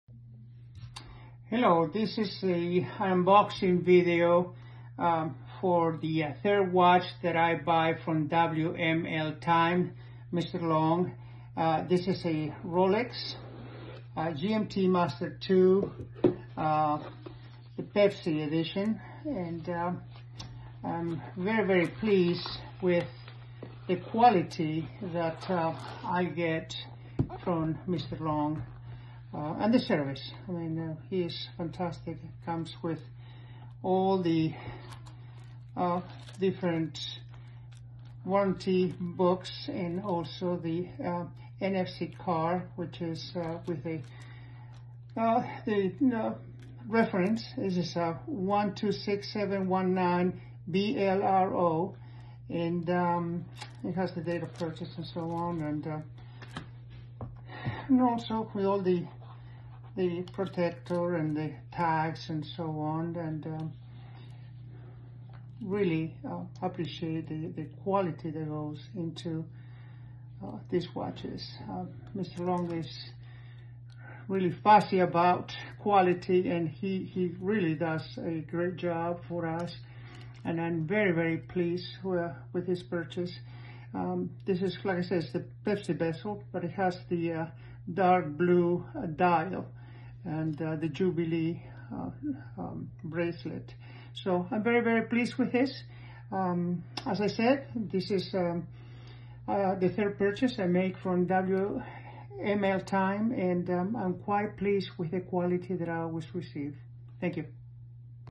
Special GMT Master II in Jubilee bracelet with blue dial
This is my fourth watch purchased from Mr.loong because I really like the jubilee bracelet,So I asked Mr.loong to help me change the bracelet